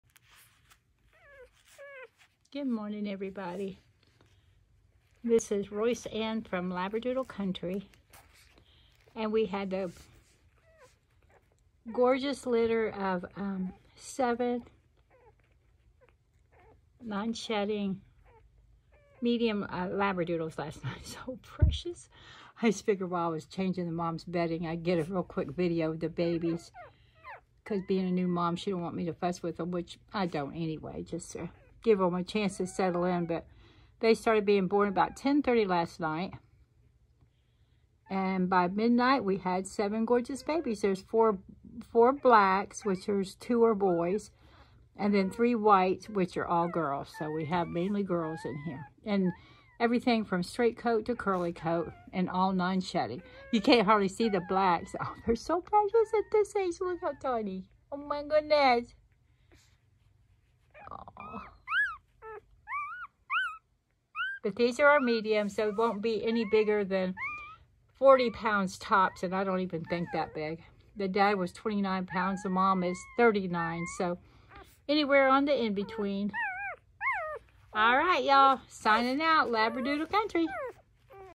October 1, 2022
Our first litter of Moyen / medium Labradoodles, 29 to 39 pounds full grown